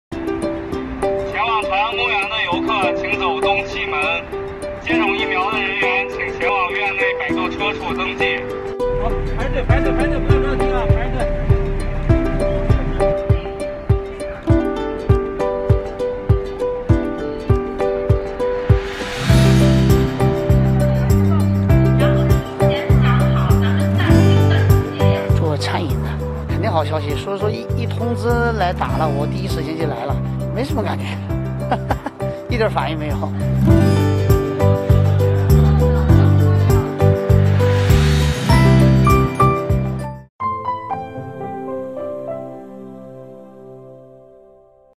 Beijing starts mass immunization against COVID-19
Beijing began mass immunization against COVID-19 on Jan. 1, 2021.
The key groups include frontline customs inspectors of imported cold-chain goods and personnel working in the overseas and domestic transportation sectors.
No serious side effects have been observed among the vaccine receivers.
"I'm engaged in catering services. Mass immunization against COVID-19 is good news, so I came here to receive inoculation soon after I was informed. There is no special reaction after the vaccination," said  a vaccine receiver surnamed Ren.
Beijing has vaccinated more than 73,000 people in the two days since the mass immunization began on Friday.